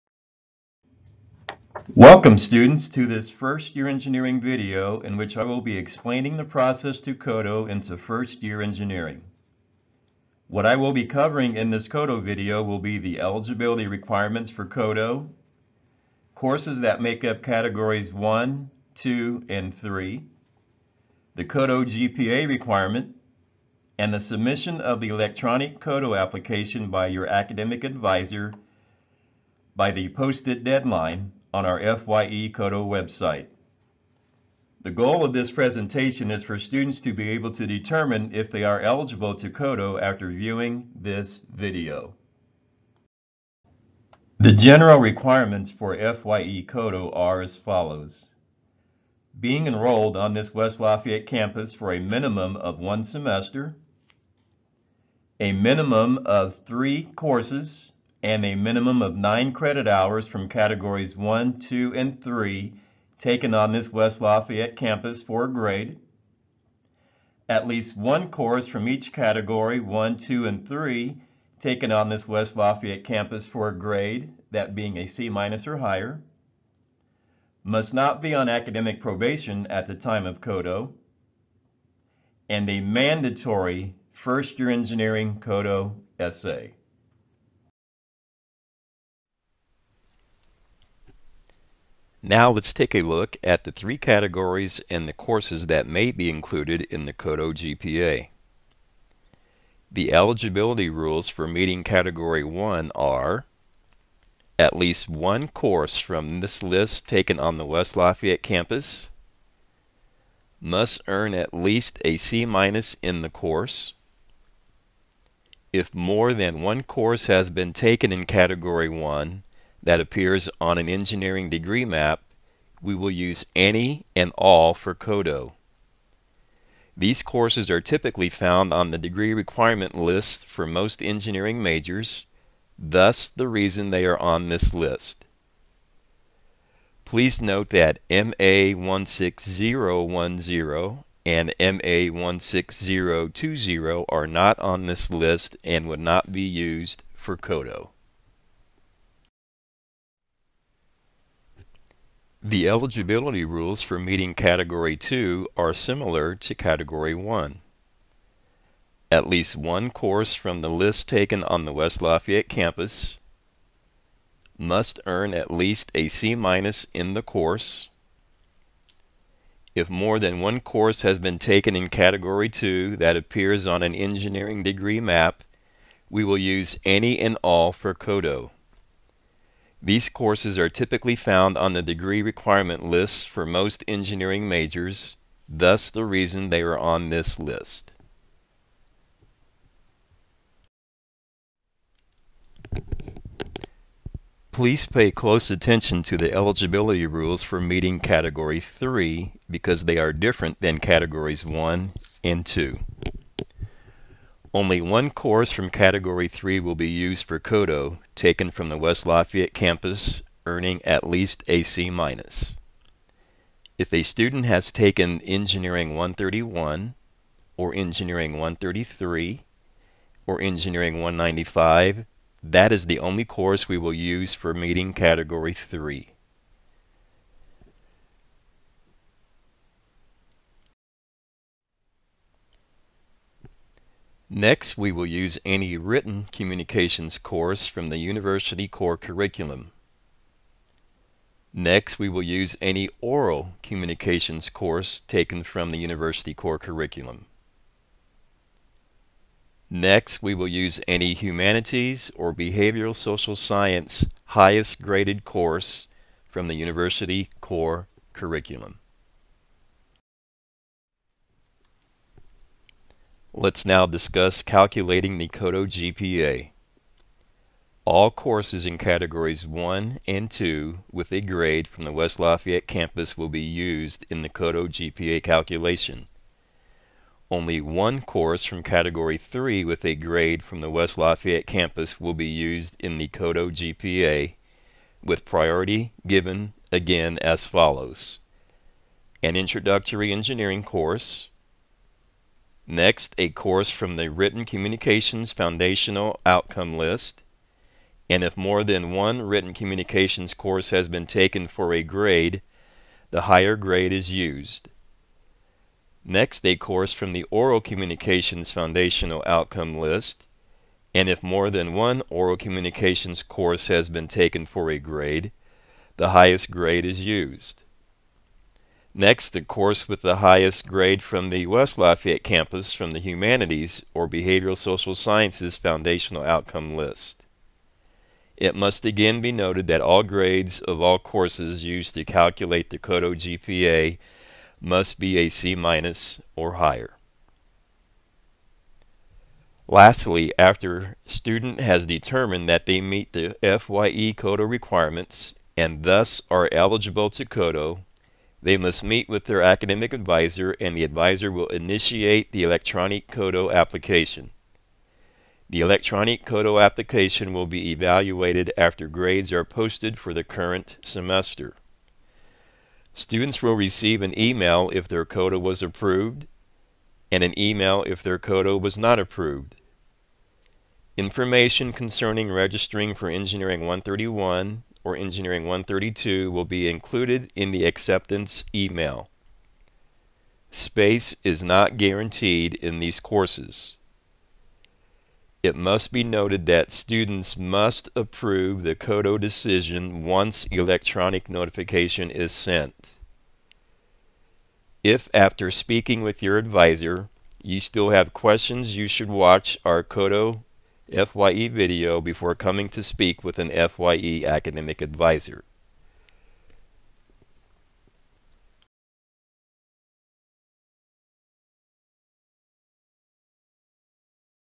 First-Year Engineering CODO Requirements Video Update
Updated video released on 11/17/20: This brief video introduces the Purdue University First-Year Engineering (FYE) Change of Degree Objective (CODO) process and requirements.

NOTE: Students are no longer required to write an essay as referenced in the video. Instead, students should complete the short answer questions using the link found on the First-Year Engineering CODO webpage under 'Process'